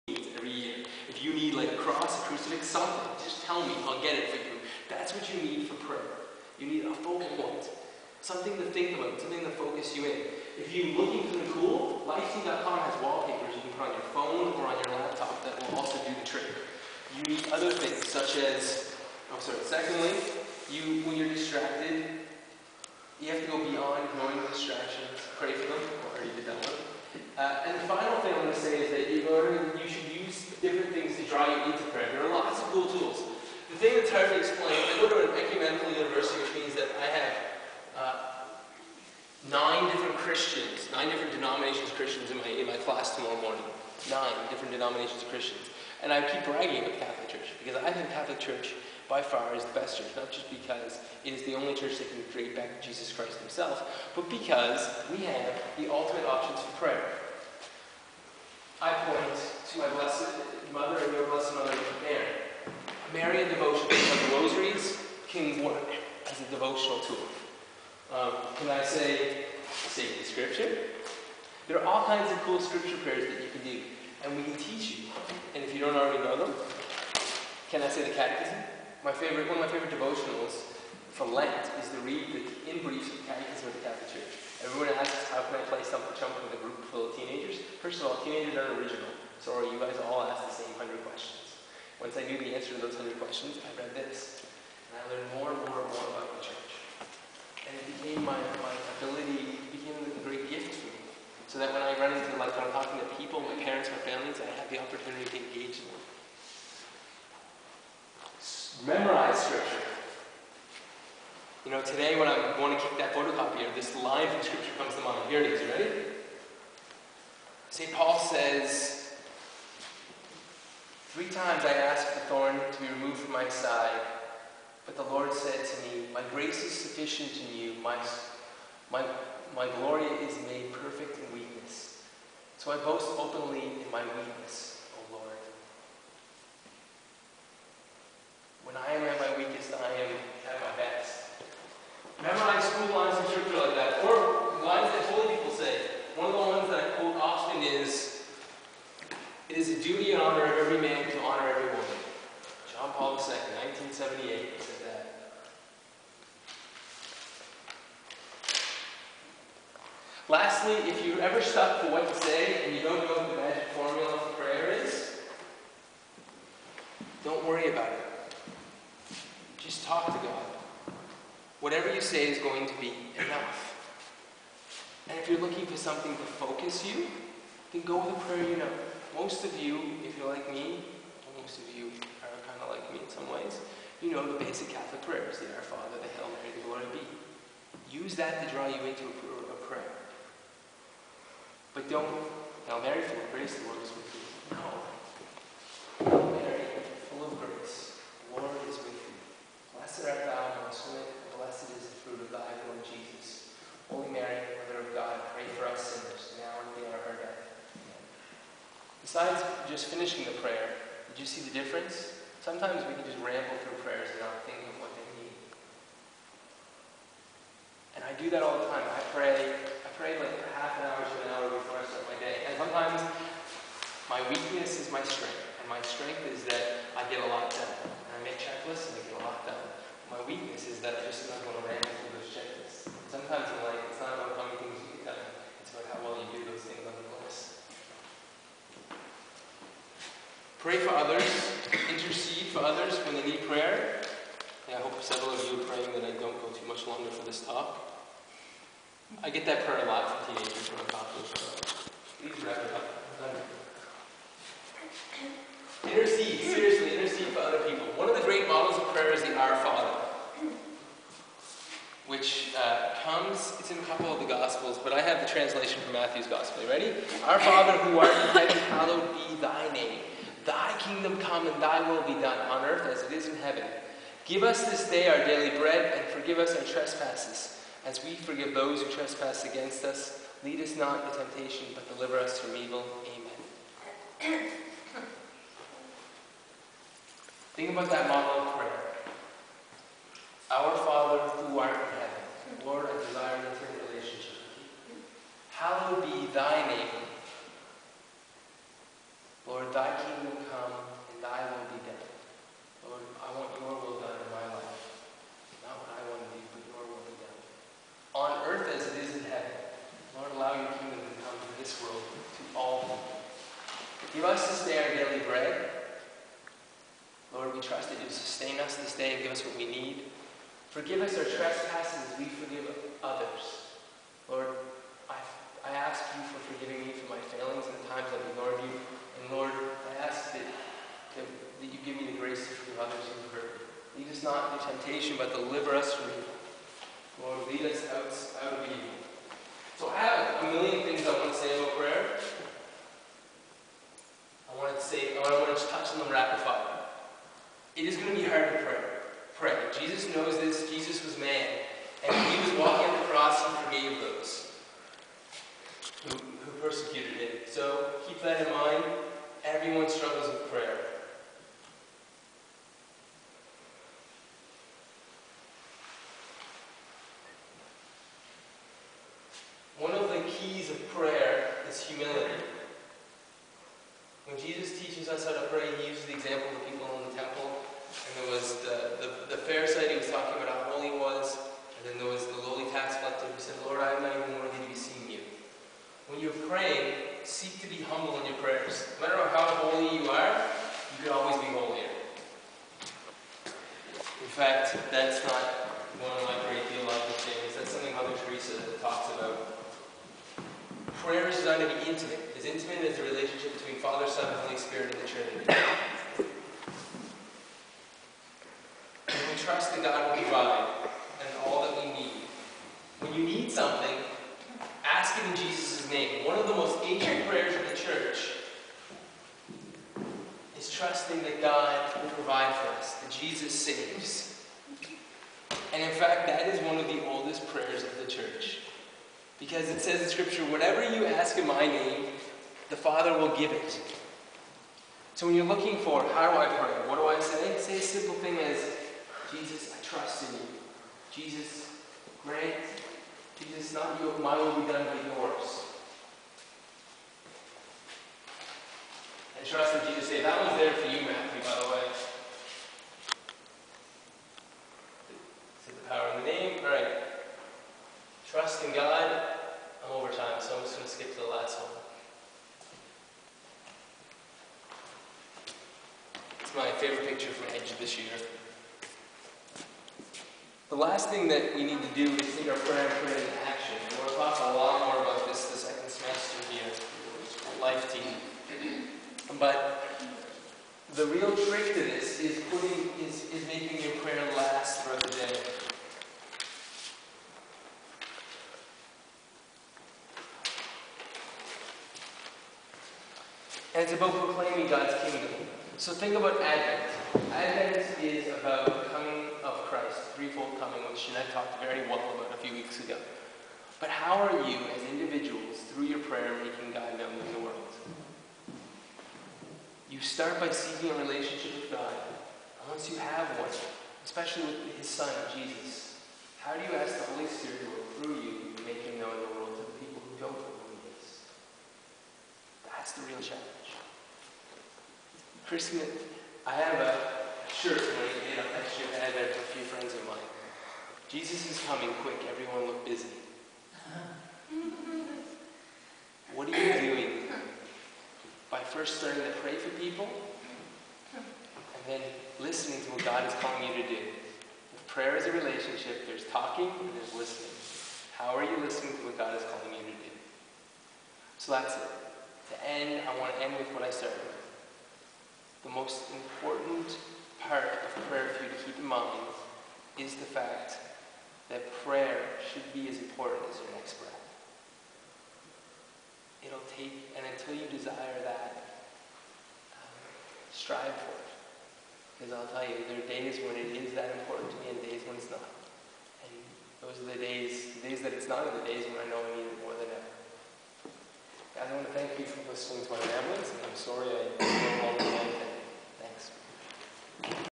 Connected Talk 2
A Talk on prayer by Brad, for Lt Halifax on Dec 12 2010.  Based on the Connect Life Night by LIFE TEEN Inc.